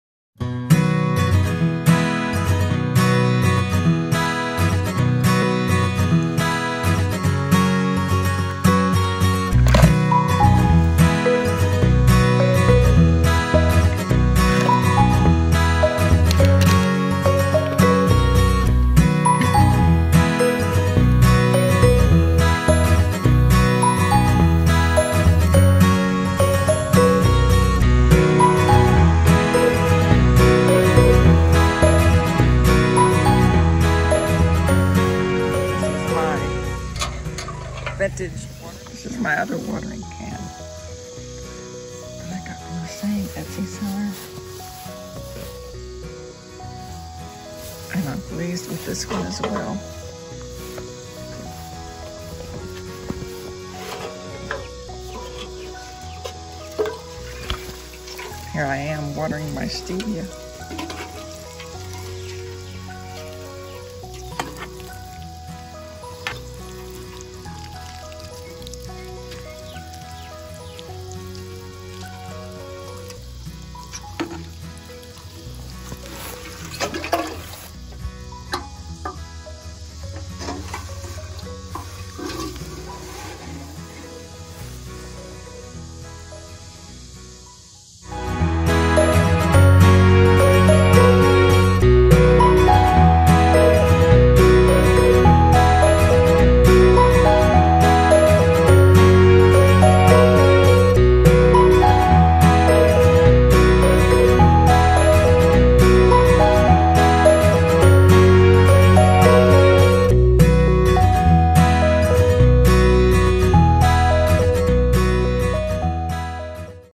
Vintage Watering Cans Video
Showing you some pictures and video of my 2 favorite vintage watering cans from Etsy Seller at DeMarsVintage. Wonderful! Just love!